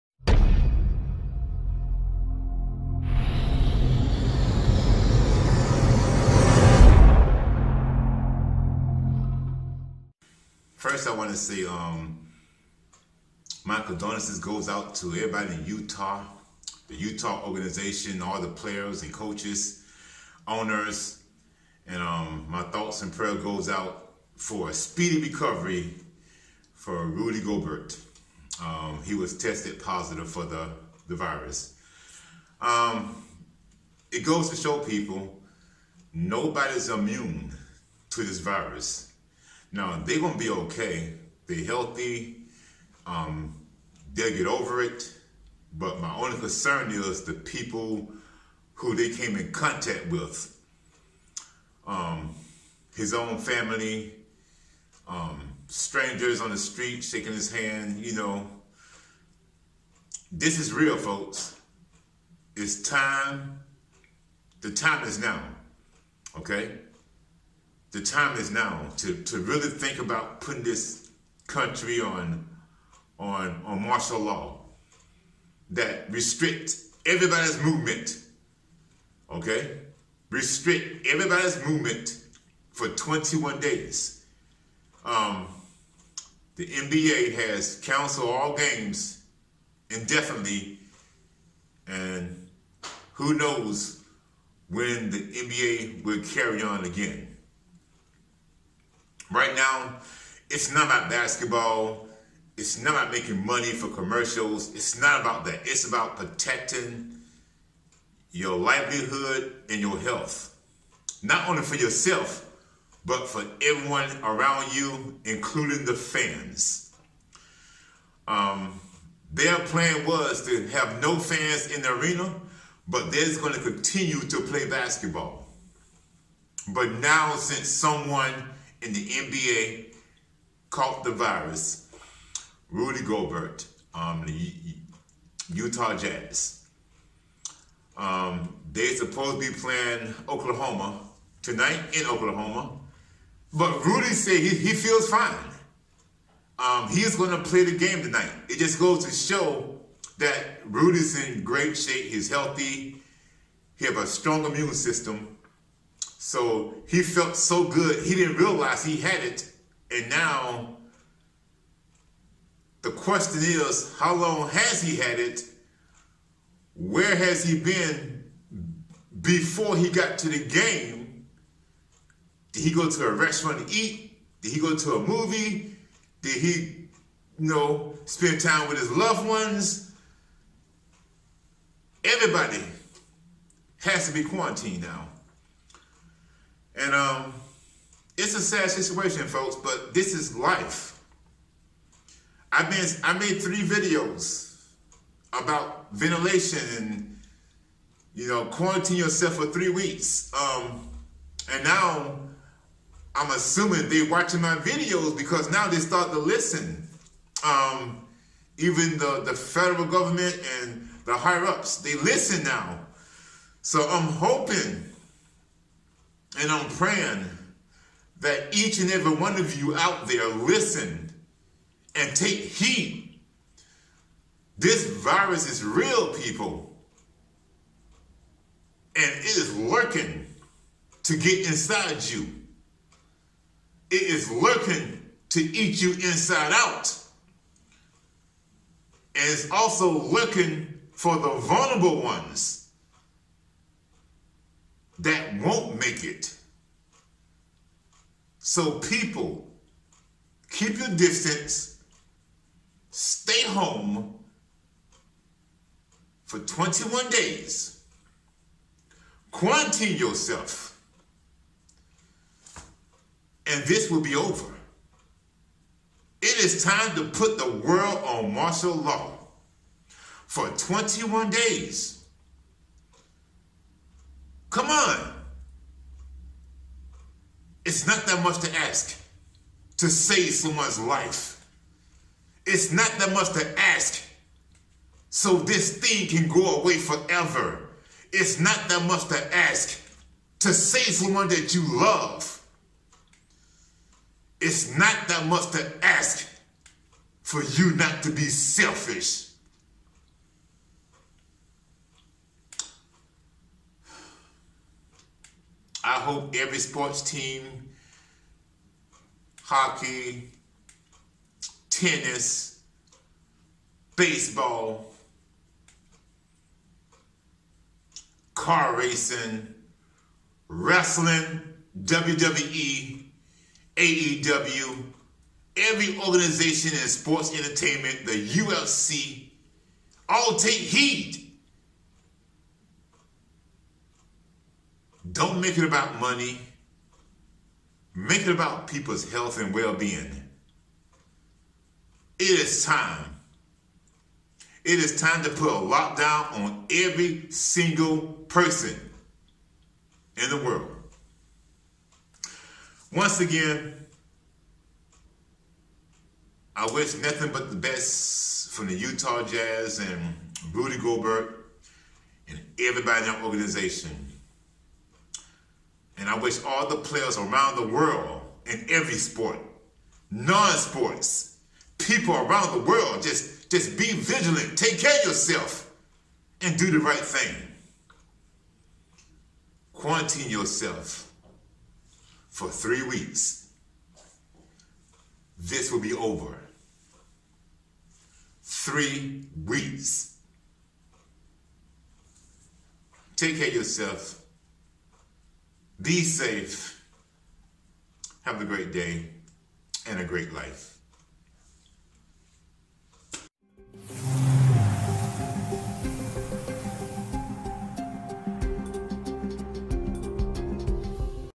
Breaking News: NBA Suspends Season Indefinitely
nba player test positive